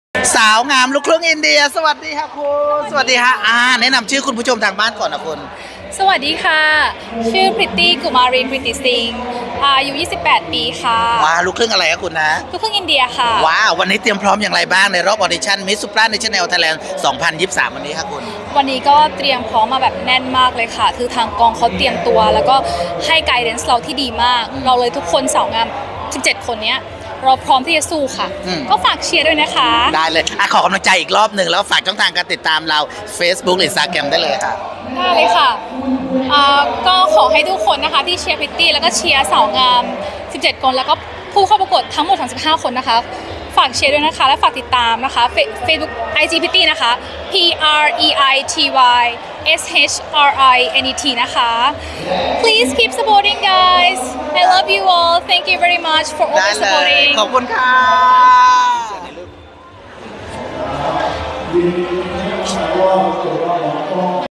ผู้เข้า​ประกวด​ Mister​ Supranational​ Thailand​ 2023​รอบ AuditionMiss & Mister Supranational
The SaM.com    ผู้เข้า​ประกวด​ Mister​ Supranational​ Thailand​  2023​
รอบ Audition
Miss & Mister Supranational Thailand 2023
SHOW DC ลาน ATRIUM ชั้น 1 Bangkok
14 มีนาคม 2566

Photo MissAngie

Miss Angie Beauty Queen Page
Havilahs.com  Hay Har
Thesam Etnews